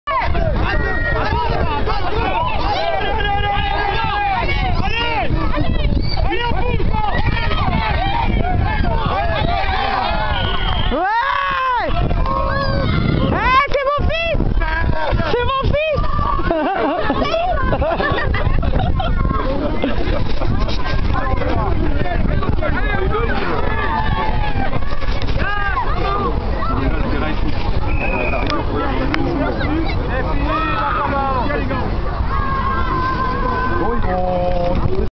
lucas rugby
c'est mon fils!!! tournoi ogs .6 essais  marqué ce jour là!!!